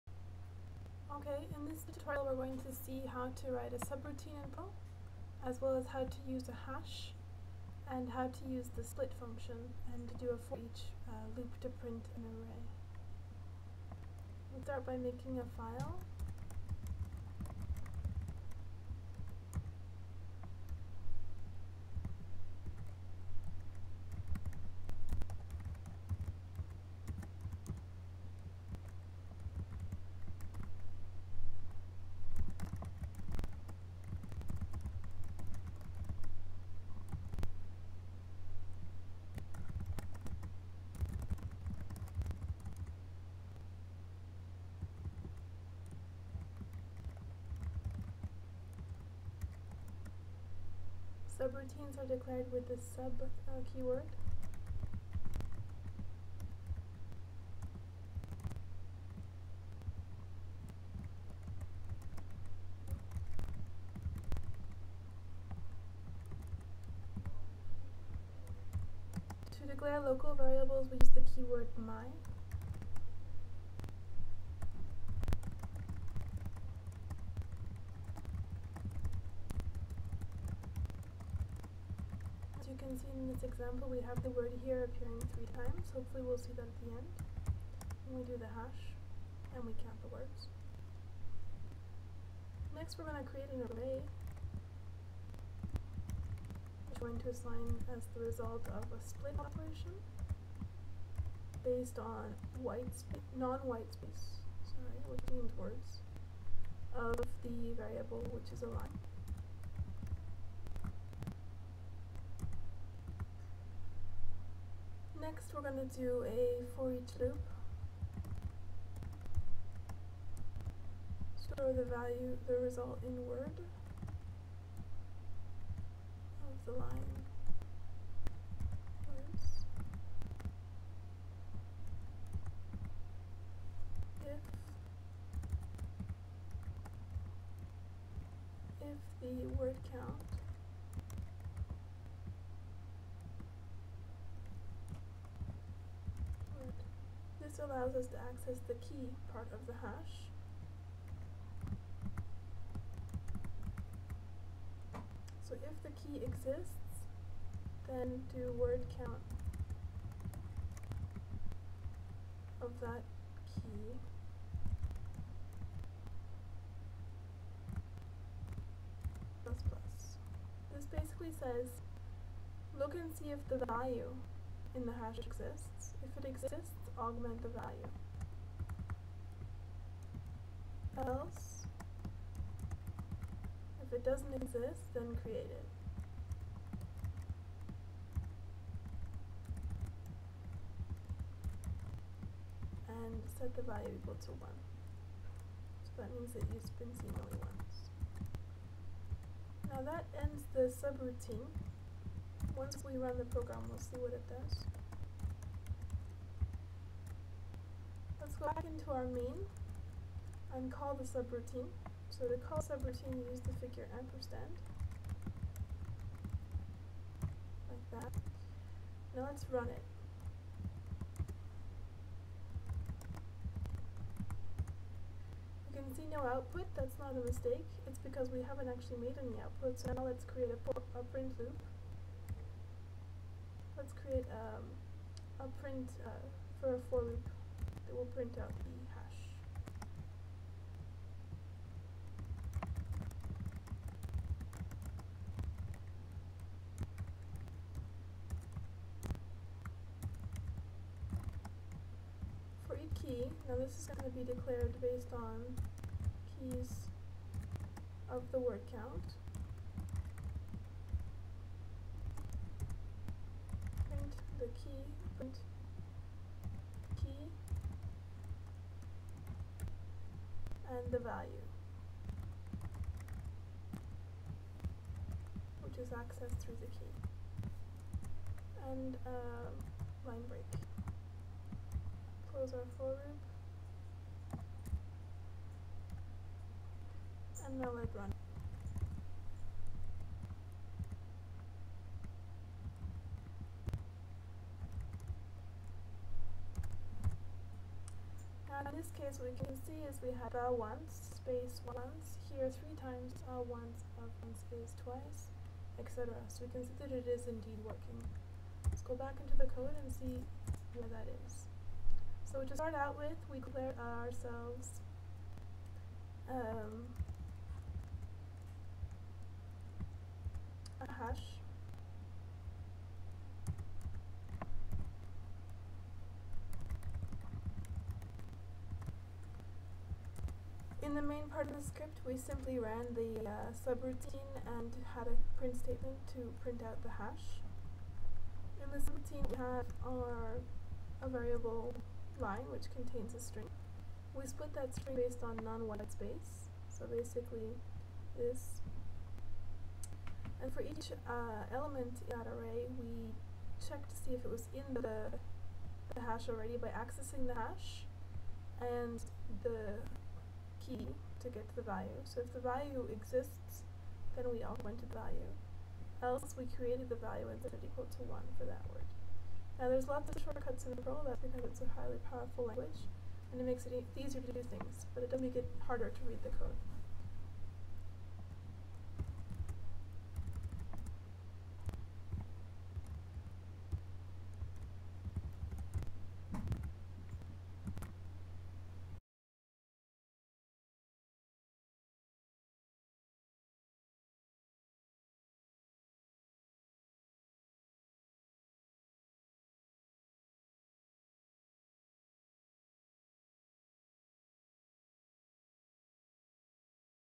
Perl script to do a word count (subroutines, hashes, foreach array, split)
This is a short perl script that counts words in a string.

The script illustrates the use of subroutines, hashes, foreach loops on arrays and the split command.